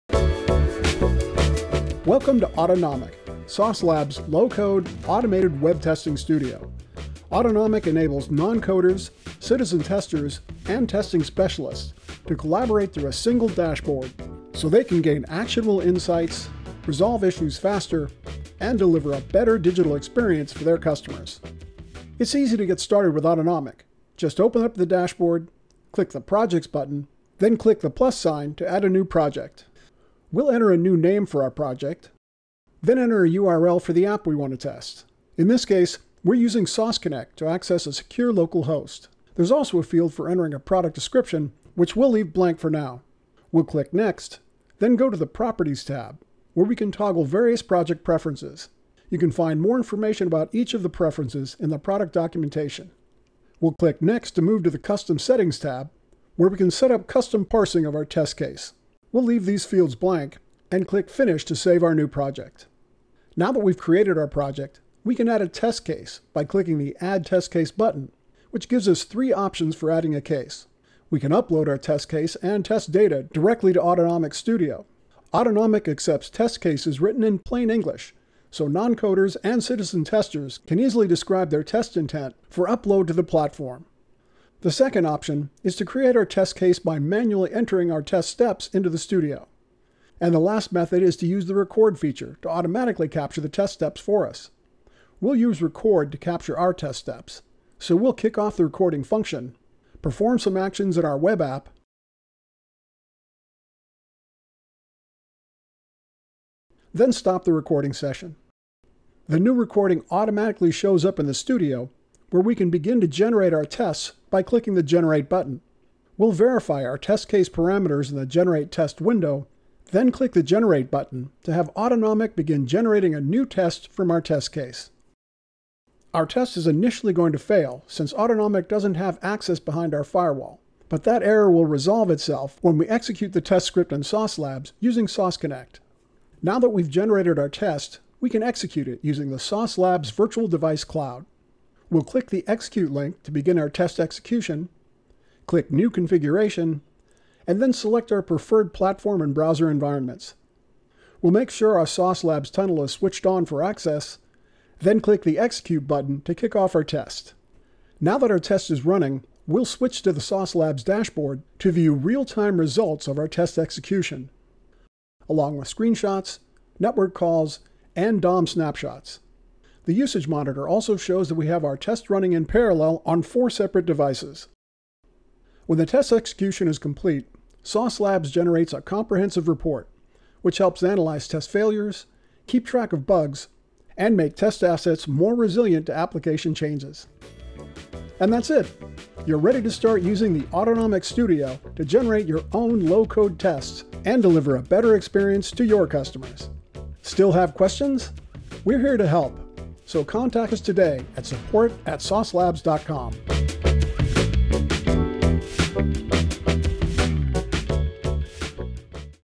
Low-code Automated Web Testing with Sauce Labs
Get started with low code automated web testing on Sauce Labs for any website and web application. Plan, design, execute, schedule, and monitor automated low-code web tests in one place.

Make testing faster, more scalable and painless. Generate codeless tests or use plain English to write and automate tests in minutes that capture your intent. Then AI-driven automation generates scripts with self-healing maintenance.

Design automation flows visually by representing anything that happens in a browser: clicking elements, typing text, and much more.

Centralize testing quality and analytics for any browser or mobile web technology, product, driving quality and critical feedback across all products.